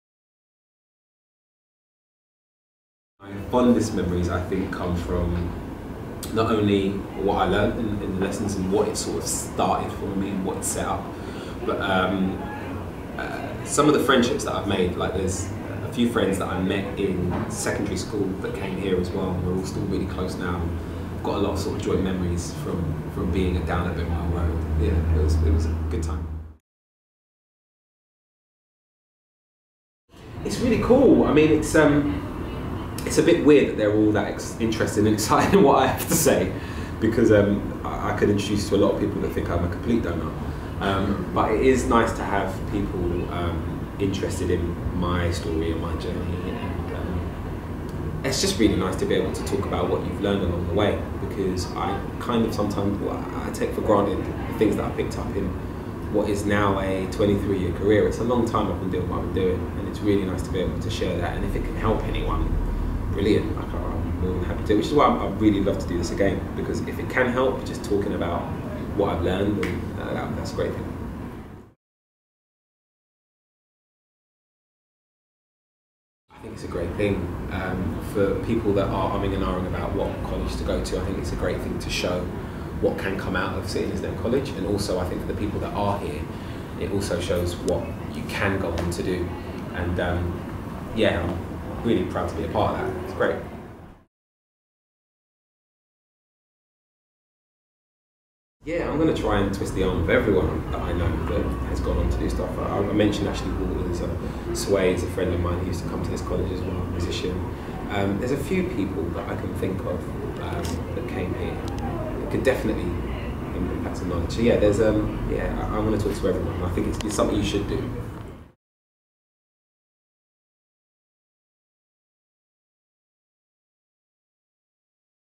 Reggie Yates Visit | City and Islington College, London
BBC presenter, actor, writer and filmmaker, Reggie Yates, a former City and Islington College A Level student, has visited the College to inspire its current crop of media students.
Despite being just 31, Reggie who is Islington born and bred, has worked in the entertainment industry for over 20 years. He studied A Levels in Media, Art and Graphic Design between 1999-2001 at our Sixth Form College, and before that attended Central Foundation School for Boys’ near Old Street.
Watch what he had to say and follow these links to find out more about studying at our Sixth Form College and signing up up to our Candi Alumni